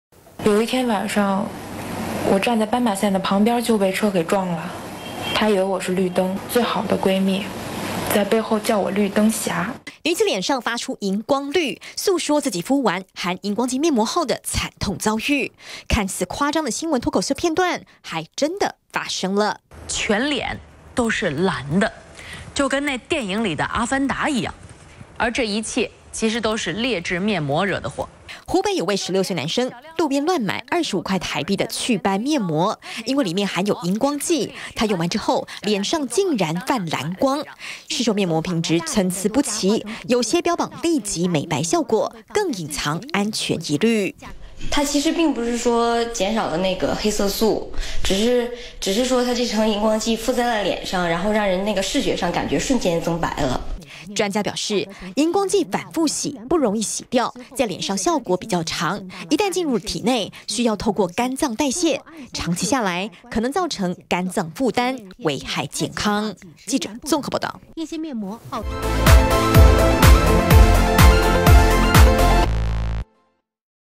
20160627中天新聞 陸男敷螢光劑面膜 臉泛藍變阿凡達
市售面膜品質參差不齊，湖北有位16歲男生，亂敷大約台幣25塊的劣質面膜，結果裡頭含有螢光劑，他敷完後，在特殊燈光照射下，臉竟然泛藍光，宛如電影中的阿凡達，嚇了一大跳。